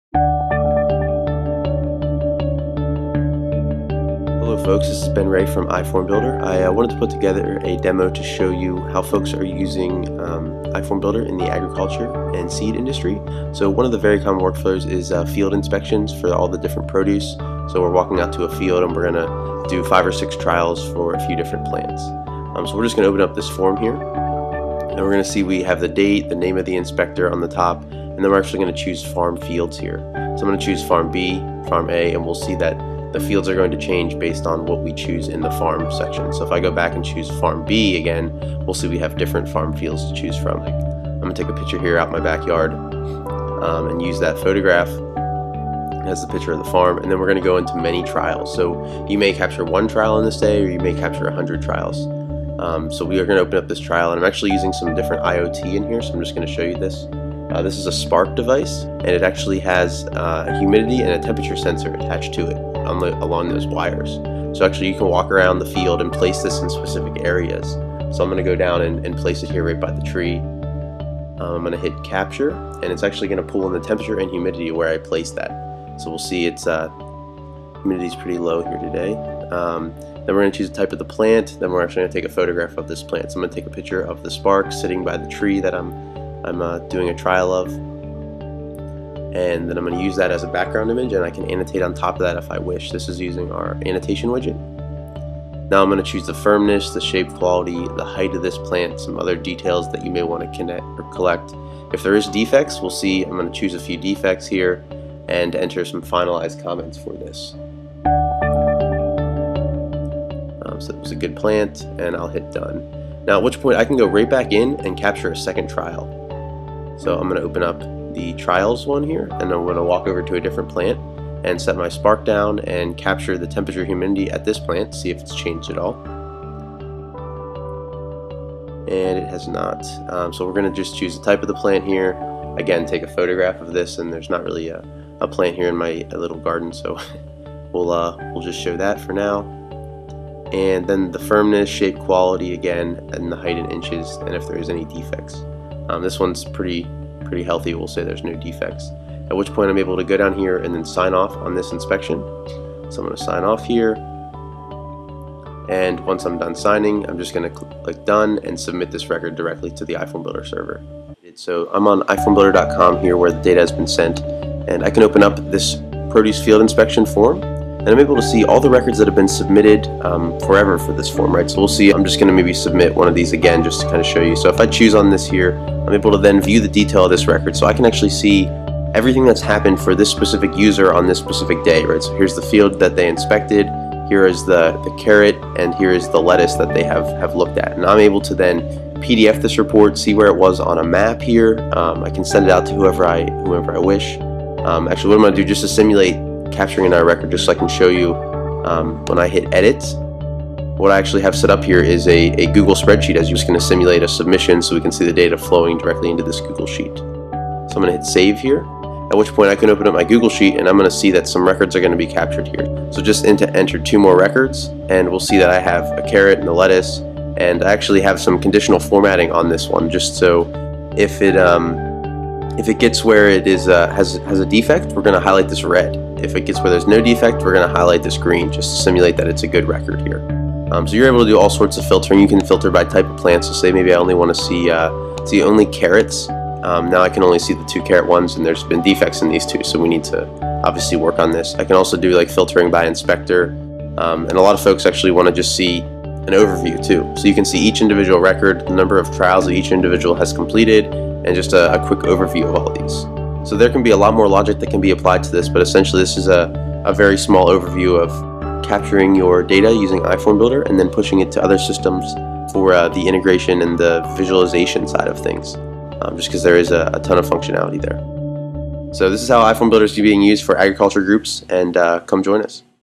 iFormBuilder for Agriculture: Field Trial Solution
This video demonstrates an end to end solution using iFormBuilder Mobile Forms to conduct field trials and Google sheets to report upon the data.